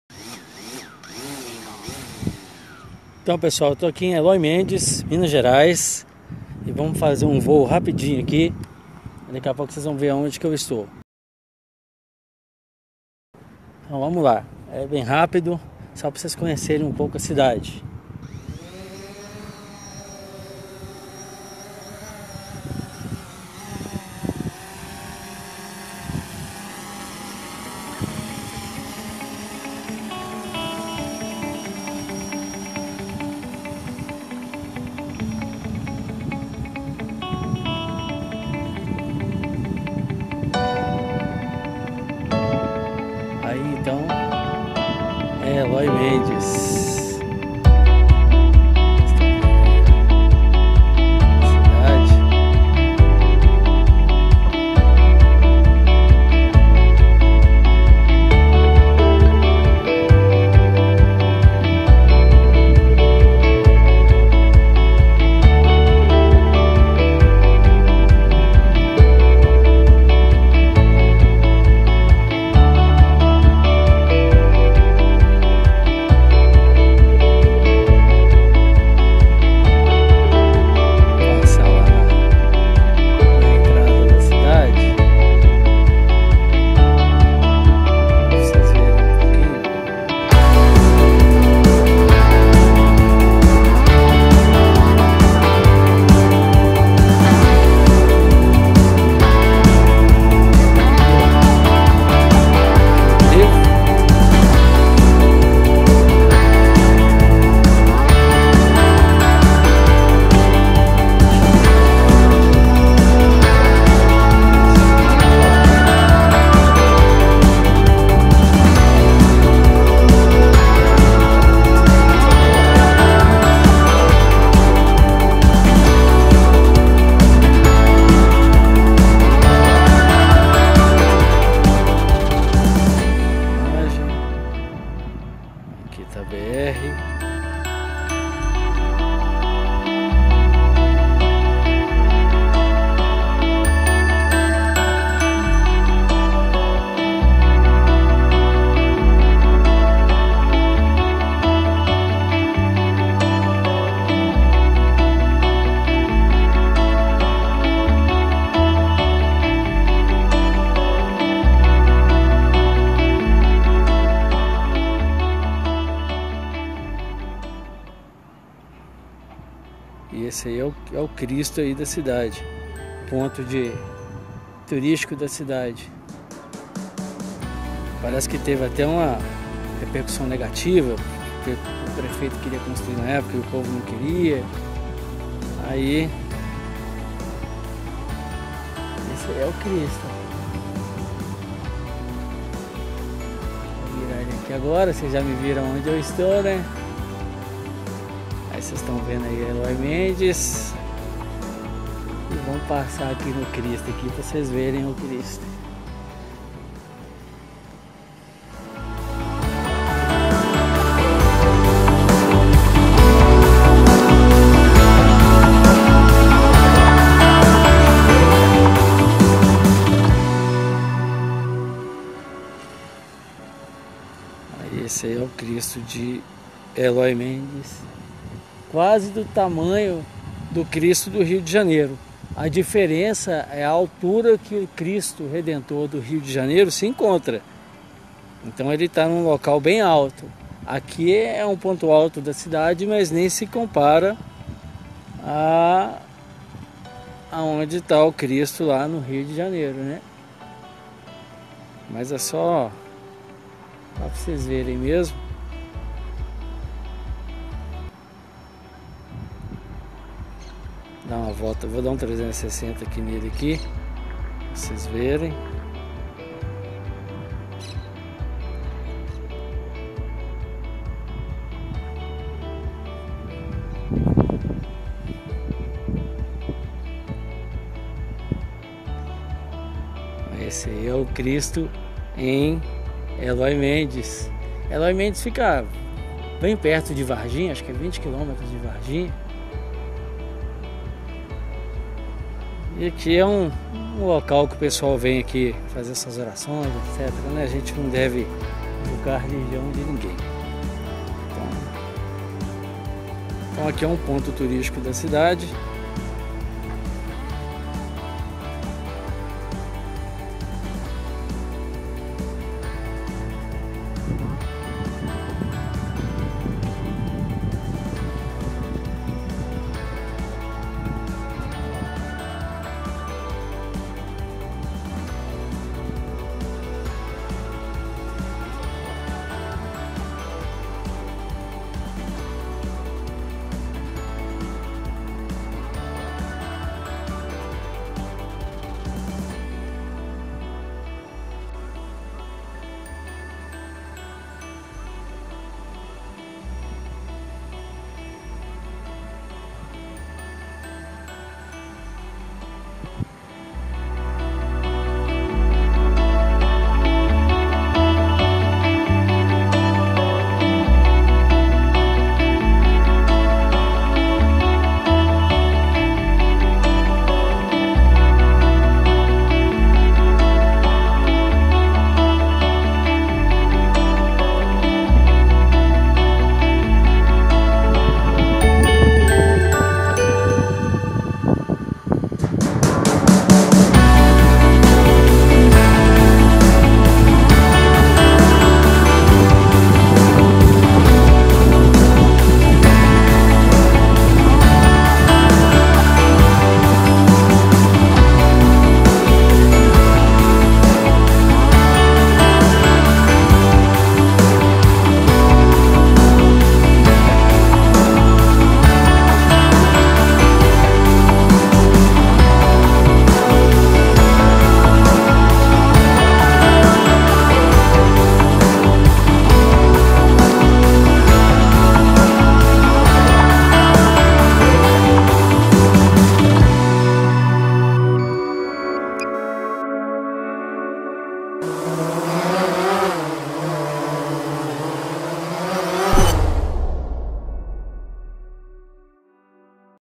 Videos Drone em Elói Mendes Minas Gerais e Cristo Redentor . 4K
Elói Mendes, é um município brasileiro do estado de Minas Gerais. Pertence à Mesorregião do Sul e Sudoeste de Minas e à Microrregião de Varginha. Wikipédia.

Vídeos Drone